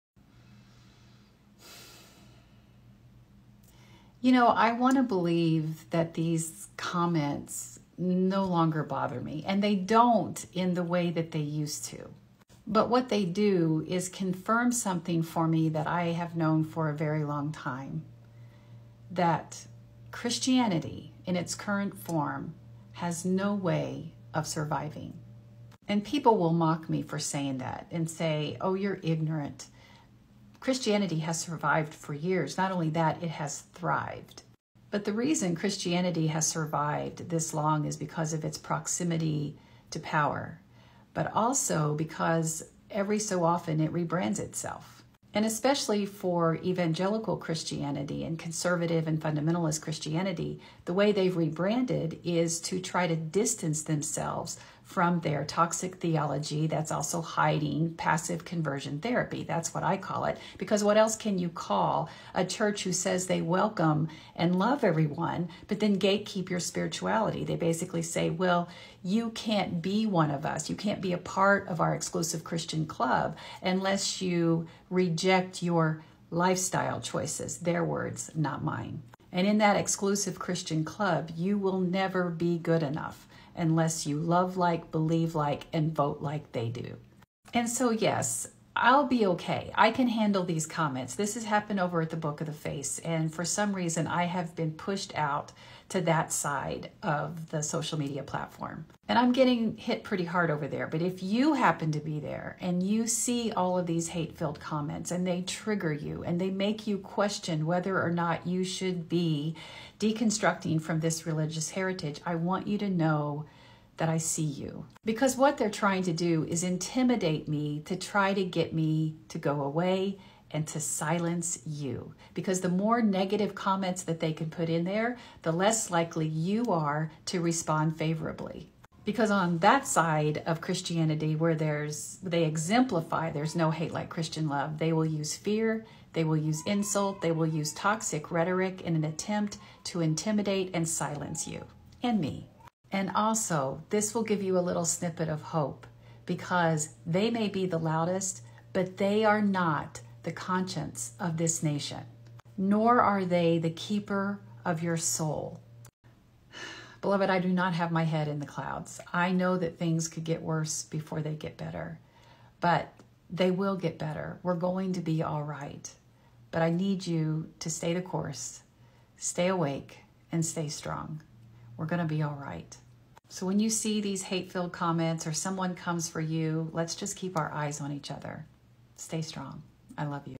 Responding to hateful comments...
I will say this again...
Christianity in its current form has no way of surviving.
Stay strong, and let's keep our eyes on each other 💜